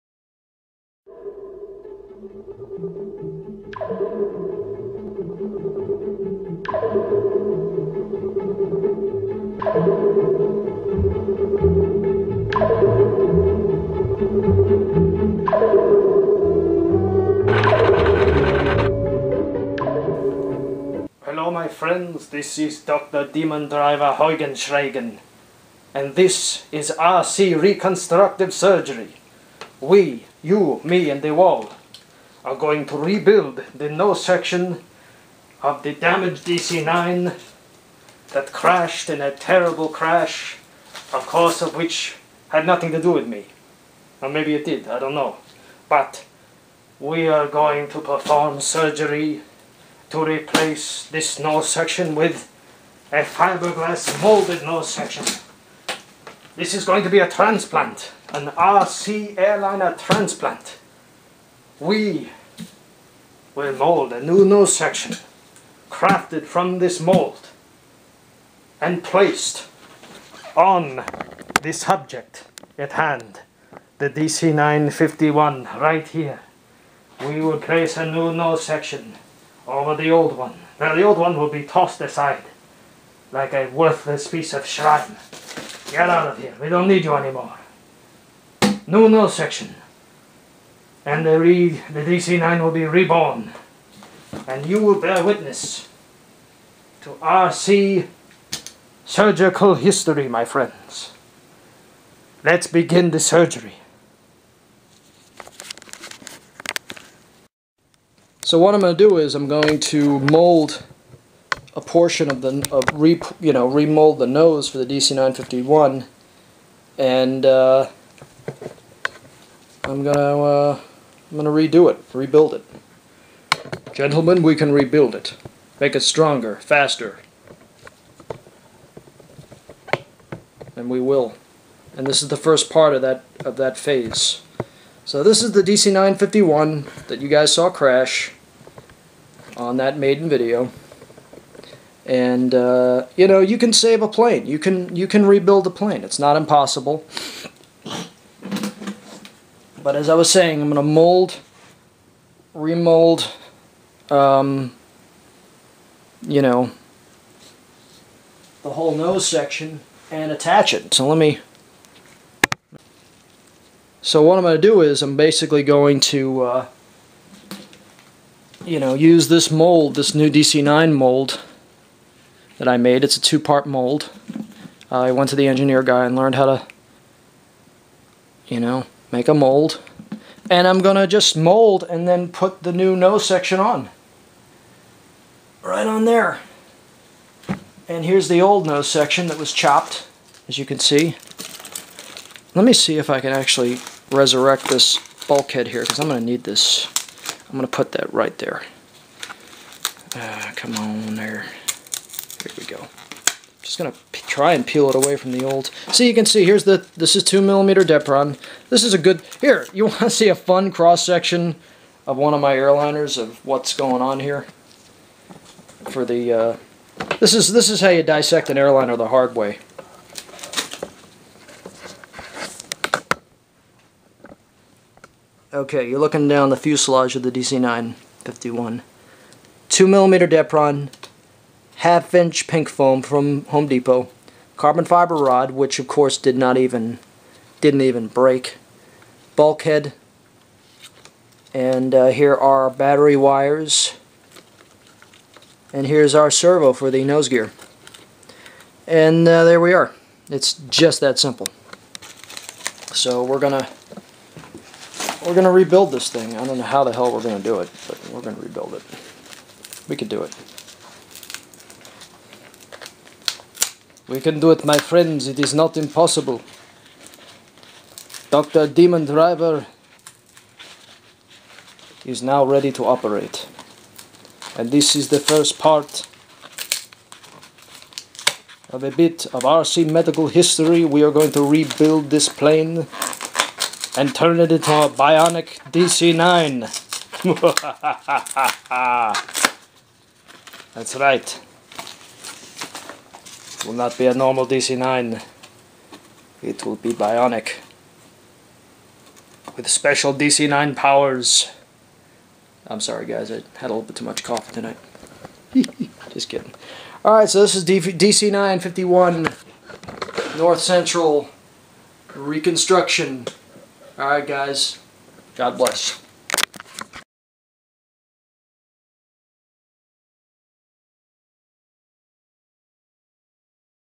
DC-9-51 RC Surgery with DemonDriver Part-1
Hi DemonDriver here and this is RC reconstructive surgery on the North Central DC-9-51! I'm going to "chop' of the old nose section & replace it with a new molded fiberglass nose section. . . enjoy!